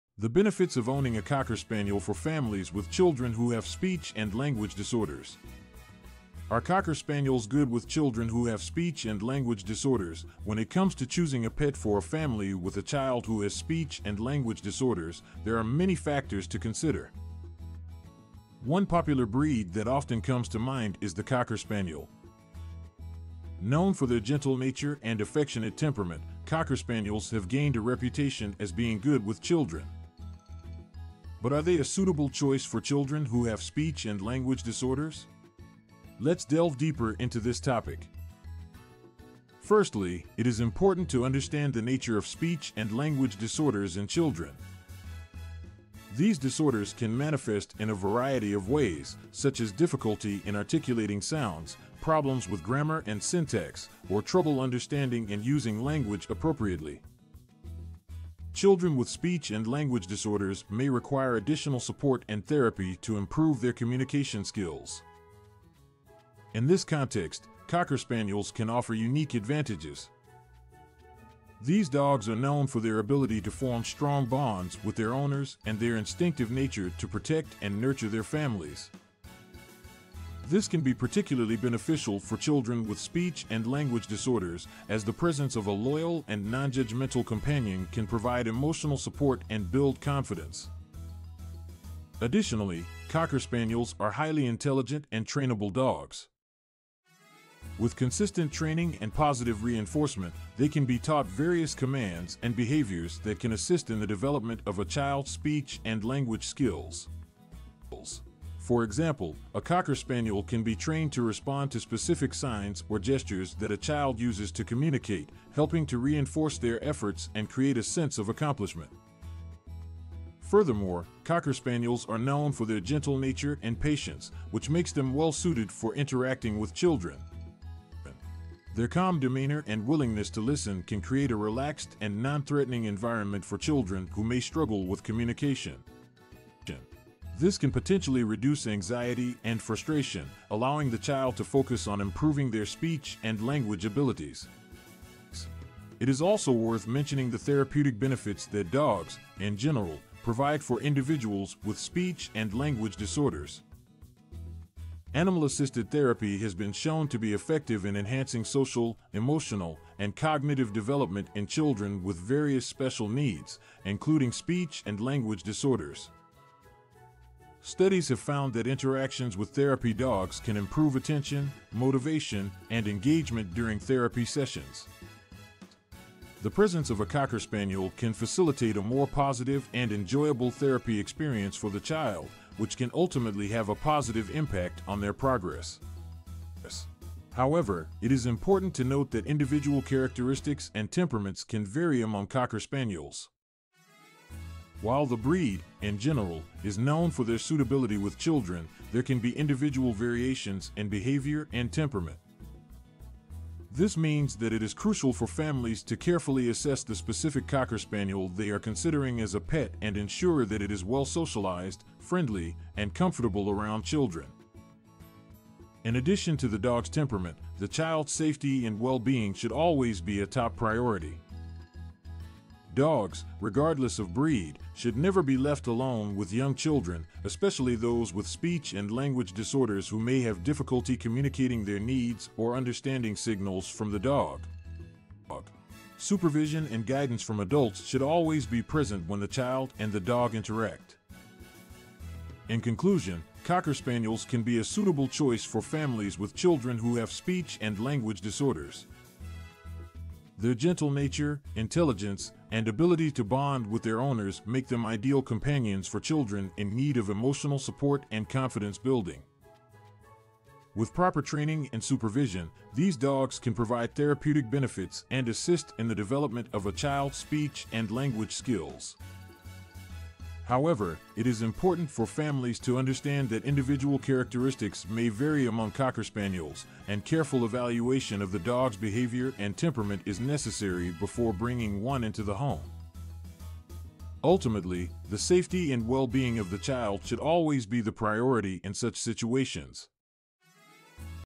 Calming Techniques to Help Alleviate Anxiety in Cocker Spaniels
Cocker Spaniel Anxiety: Calming Techniques

Cocker Spaniels are beautiful and popular dog breeds known for their long, flowing coats and gentle personalities   However, just like any other breed, these dogs can occasionally suffer from anxiety   If you are a Cocker Spaniel owner and notice signs of anxiety in your furry friend, it is important to address the issue promptly and effectively   In this article, we will explore some calming techniques that can help alleviate anxiety in Cocker Spaniels

1 Create a Safe Space:
Anxiety in dogs can often be triggered by loud noises or unfamiliar surroundings
00:00 Calming Techniques to Help Alleviate Anxiety in Cocker Spaniels